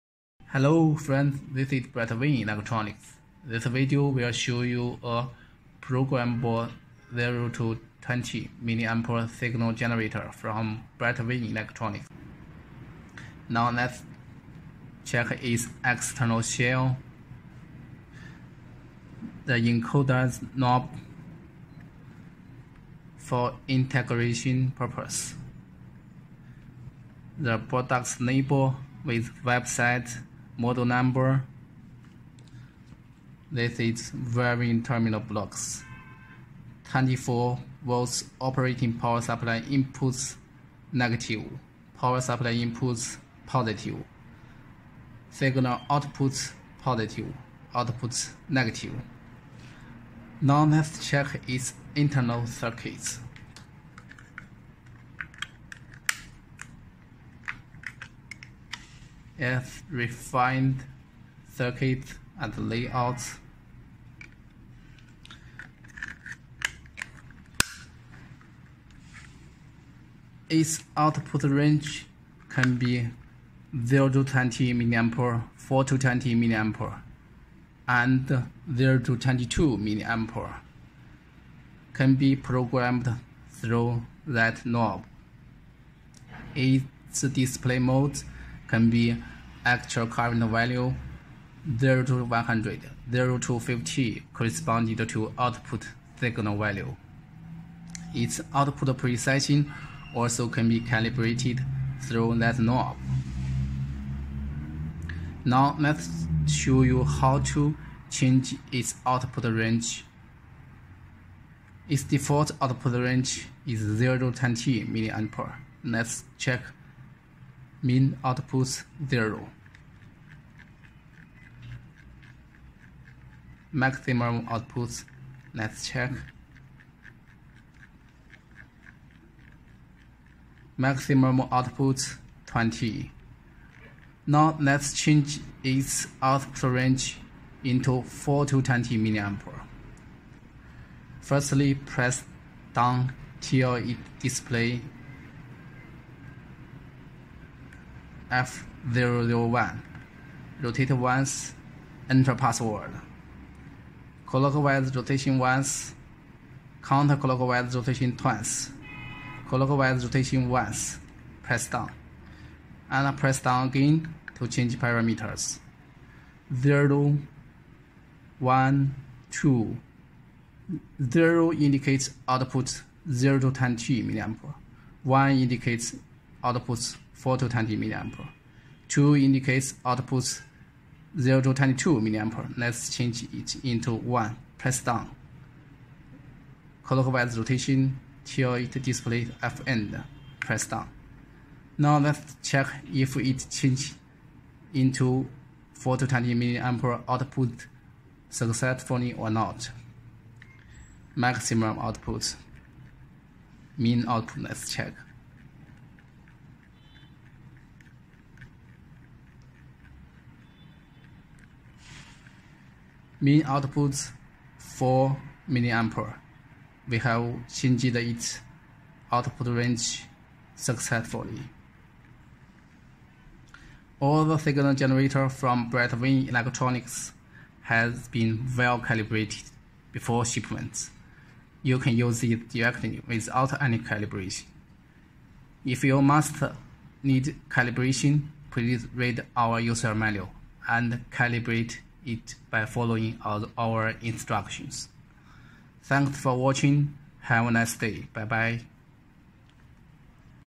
Programmable 0 20mA 4 20mA Signal Generator BRT 420G HM3
Brightwin programmable current source signal generator is an intelligent adjustable 0-20mA 4-20mA 0-22mA analog signal output panel-mounted meter. Its output accuracy is 0.5%, output signal is adjustable through precision encoders’ knob. Its output range, LED display mode, adjusting steps are adjustable and output precision can be calibrated by user. It has parameters setting auto save function in case of power off. That BRT 420G-HM3 4-20mA signal generator output range can be 0-20mA, 4-20mA, 0-22mA, display mode can be actual current value, 0-100(%), 0-50(Hz), operating power supply 15-30VDC. It is an ideal low-cost current source signal simulator or testing device.